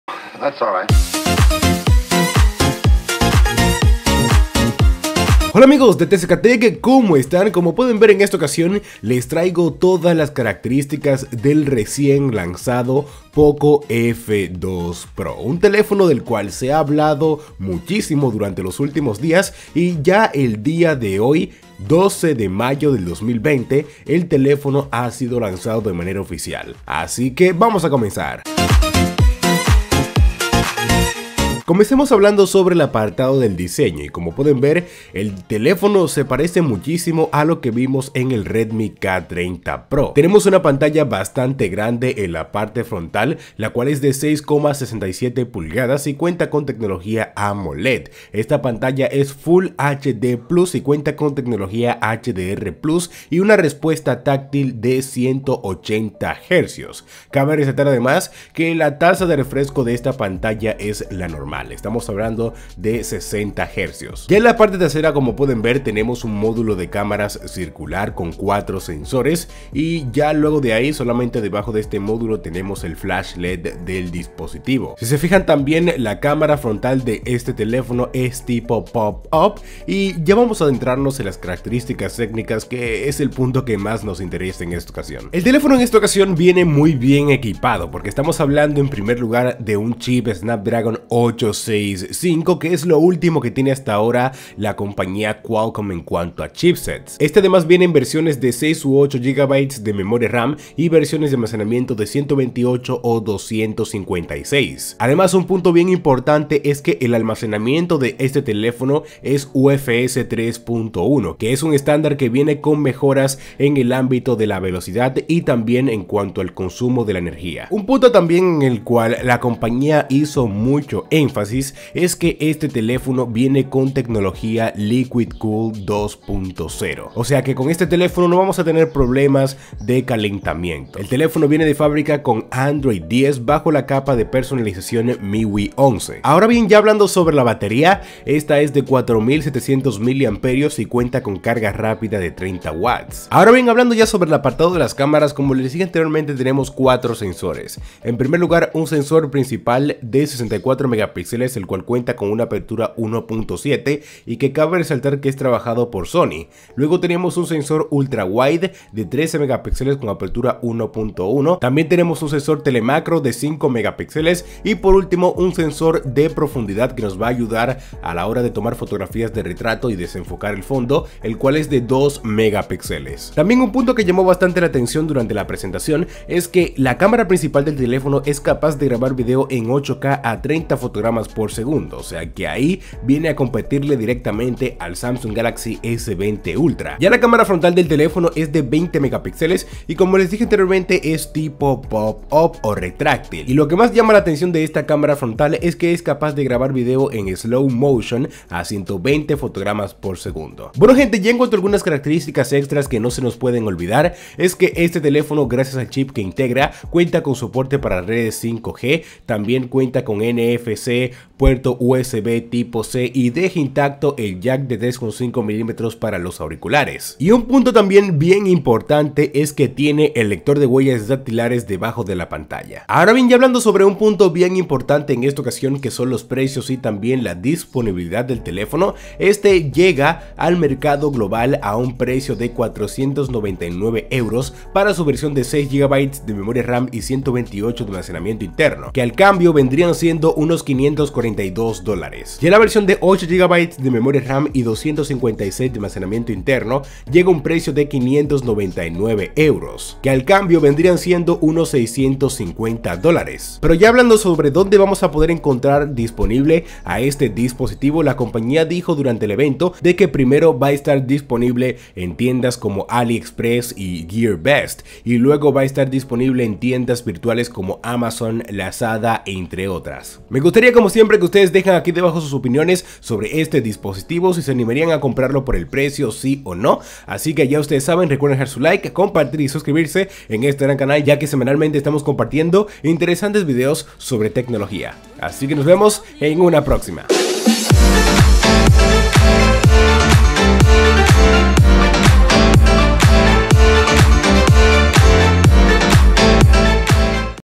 Poco F2 PRO ¡UNA VERDADERA BESTIA ECONÓMICA! | Precios y Disponibilidad
Conoce al nuevo Poco F2 Pro, una COMPLETA bestica de smartphone que llega a un precio super economico.